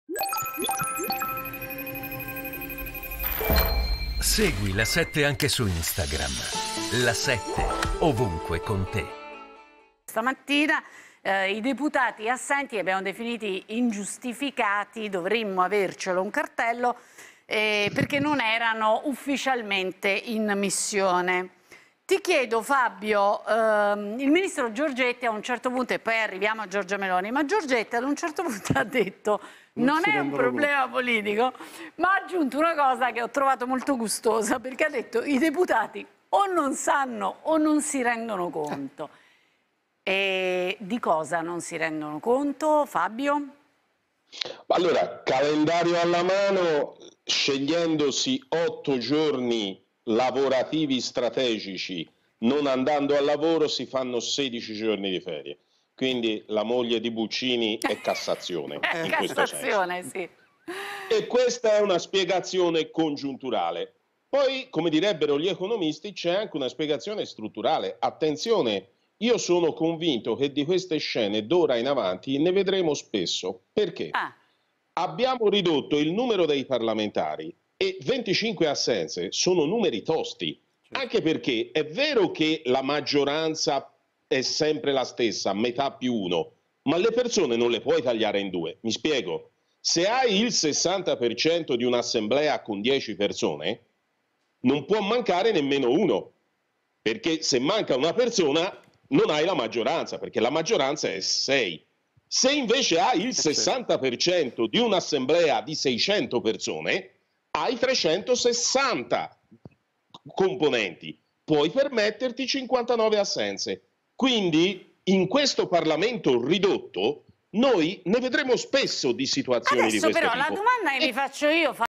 Governo senza maggioranza, Dragoni: "In futuro sarà sempre più comune"
Il commento di Dragoni sul mancato raggiungimento dei voti neccessari alla maggioranza sul DEF